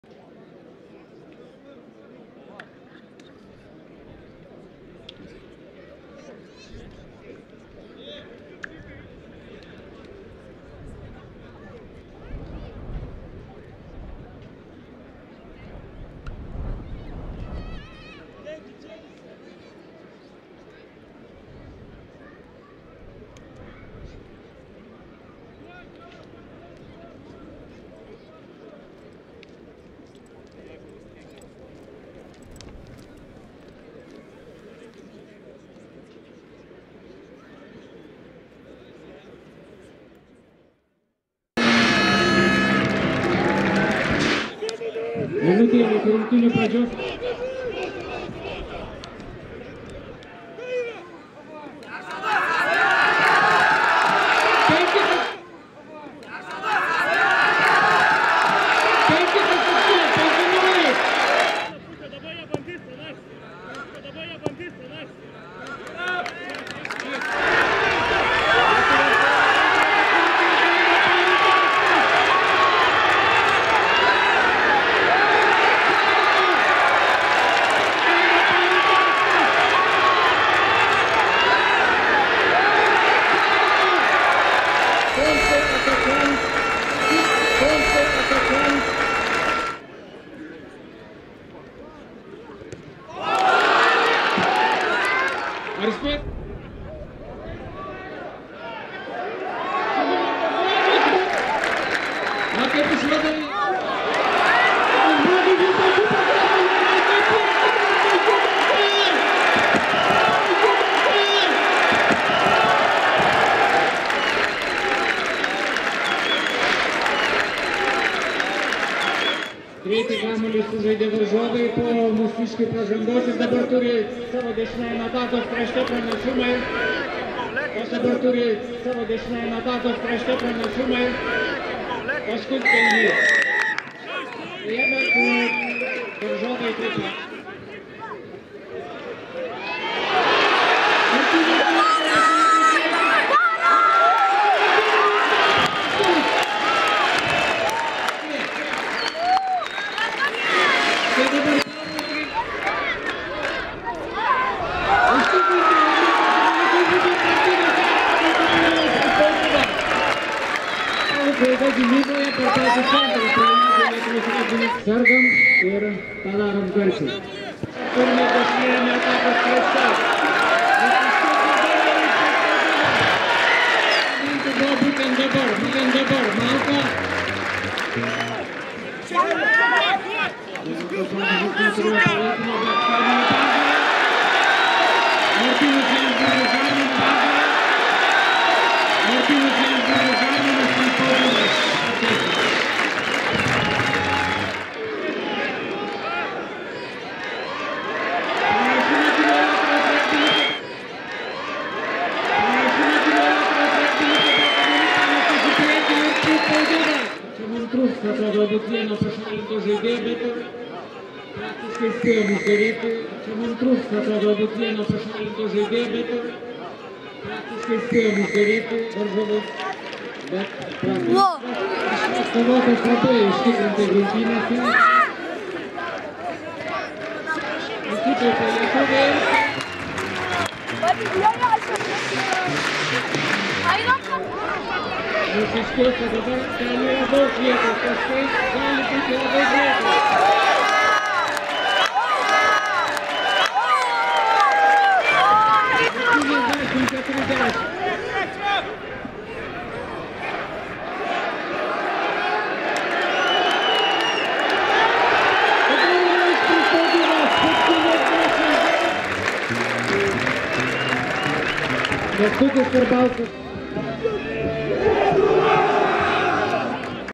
Lithuania - Malta rugby highlights
Lietuvos regbio rinktinė net 81:10 (45:5) sutriuškino Maltą ir pakilo į Europos čempionato "Trophy" divizioną.